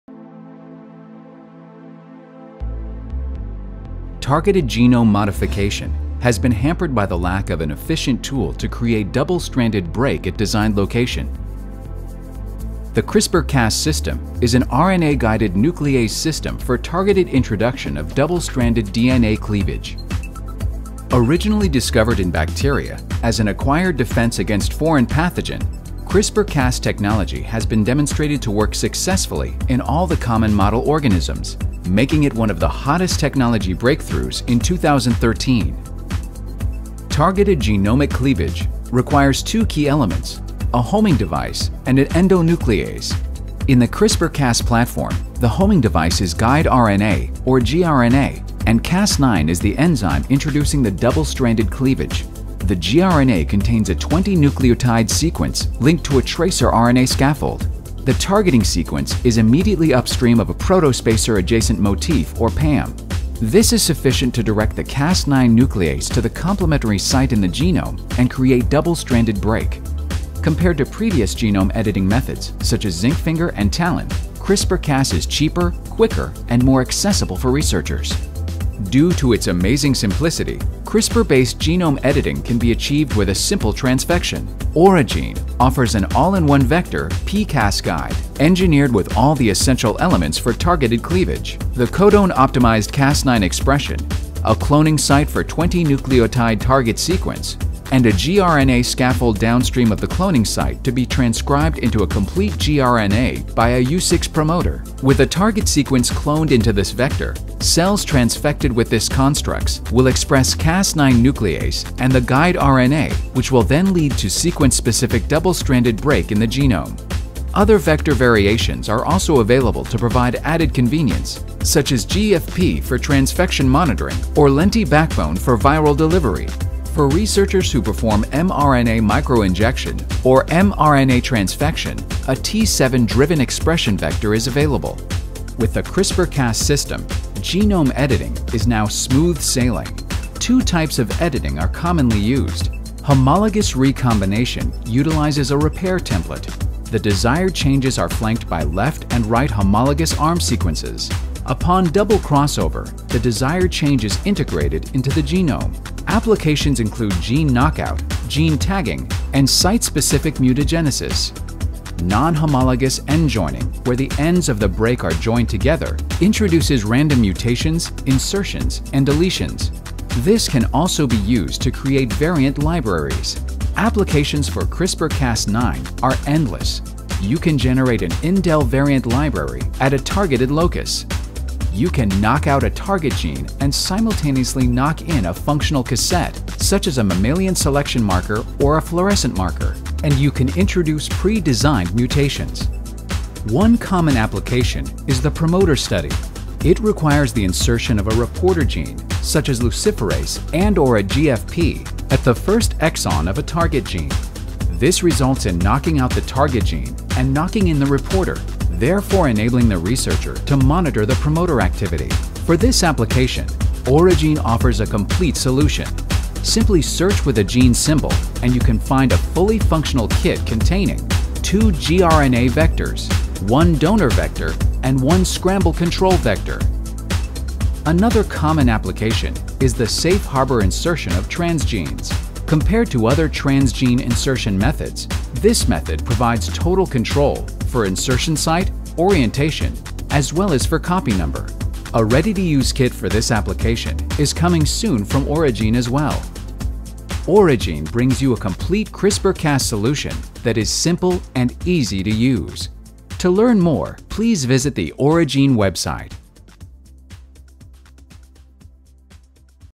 CRISPR Cas9 Explained
What is CRISPR? What is Cas9? How does CRISPR system work? What is needed to perform genome engineering in your lab? This short video introduces the basics of this novel technology, as well as genome editing/knockout products offered by OriGene.

Targeted genome modification has been hampered by the lack of an efficient tool to create a double-stranded break at a designed location. The CRISPR/CAS system is an RNA-guided nuclease system for the targeted introduction of double stranded DNA cleavage.

Originally discovered in bacteria as an acquired defense against foreign pathogens, CRISPR/CAS technology has been demonstrated to work successfully in all the common model organisms, making it one of the hottest technology breakthroughs in 2013.

Targeted genomic cleavage requires two key elements: a homing device and an endonuclease. In the CRISPR/CAS platform, the homing device is guide RNA, or gRNA and a CAS nuclease.

The gRNA contains a twenty-nucleotide target sequence immediately upstream of a Protospacer Adjacent Motif or PAM, linked to a tracrRNA scaffold. This is sufficient to direct the CAS9 nuclease to the complementary site in the genome and create a double-stranded break.

Compared to previous genome editing methods, such as ZNF and TALEN, CRIPSR/CAS is cheaper, quicker, and more accessible for researchers.

Due to its amazing simplicity, CRIPSR-based genome editing can be achieved with a simple transfection.

OriGene offers an all-in-one vector, pCas-Guide, engineered with all the essential elements for targeted cleavage.
• The codon-optimized Cas9 expression
• A cloning site for a twenty nucleotide target sequence
• And a gRNA scaffold downstream of the cloning site to be transcribed into a complete gRNA by a U6 promoter.

With a target sequence cloned into this vector, cells transfected with these constructs will express Cas9 nuclease and the guide RNA, which will then lead to a sequence-specific double-stranded break in the genome.

Other vector variations are also available to provide added convenience, such as GFP for transfection monitoring or Lenti-backbone for viral delivery. For researchers who perform mRNA microinjection or mRNA transfection, a T7 driven expression vector is available.

With the CRISPR/CAS system, genome editing is now smooth sailing. Two types of editing are commonly used.  Homologous recombination utilizes a repair template. The desired changes are flanked by left and right homologous arm sequences. Upon double crossover, the desired change is integrated into the genome.

Applications include gene knockout, gene-tagging, and site-specific mutagenesis.

Non-homologous end joining, where the ends of the break are joined together, introduces random mutations, insertions, and deletions.

This can also be used to create variant libraries.

Applications for CRISPR/Cas9 are endless.  You can generate an in-del variant library at a targeted locus.

You can knock out a target gene and simultaneously knock-in a functional cassette, such as a mammalian selection marker or a fluorescent marker.

And you can introduce pre-designed mutations.

One common application is the promoter study. It requires the insertion of a reporter gene, such as luciferase and/or a GFP, at the first exon of a target gene. This results in knocking out the target gene and knocking in the reporter, therefore enabling the researcher to monitor the promoter activity.

For this application, OriGene offers a complete solution.  Simply search with a gene symbol, and you can find a fully functional kit containing
• Two gRNA vectors,
• One donor vector,
• And one scramble control vector

Another common application is the safe-harbor insertion of transgenes. Compared to other transgene insertion methods, this method provides total control, for insertion site, orientation, as well as for copy number.

A ready-to-use kit for this application is coming soon from OriGene as well.

A ready-to-use kit for this application is now available from Origene.

Origene brings you a complete CRISPR/Cas solution that is simple and easy to use. To learn more please visit the Origene website.